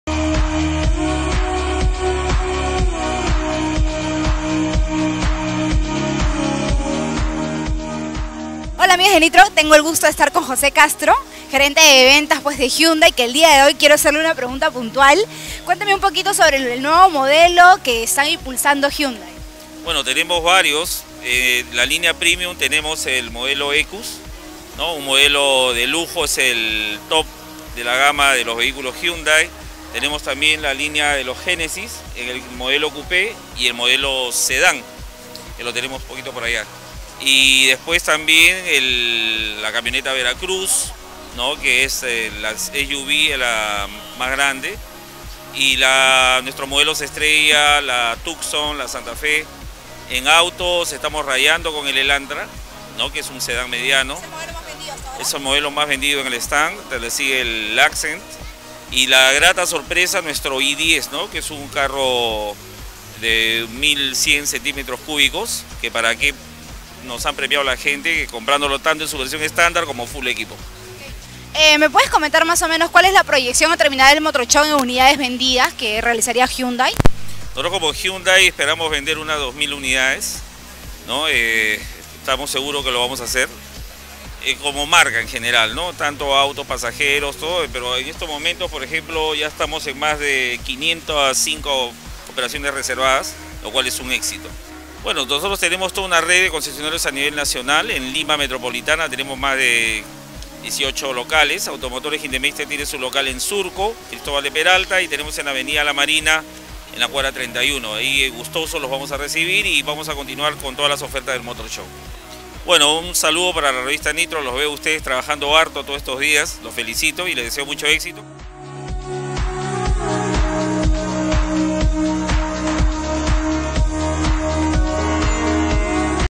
Motor Show 2010: Hyundai
Hyundai presente en el motor lima 2010 presentando sus diversos modelos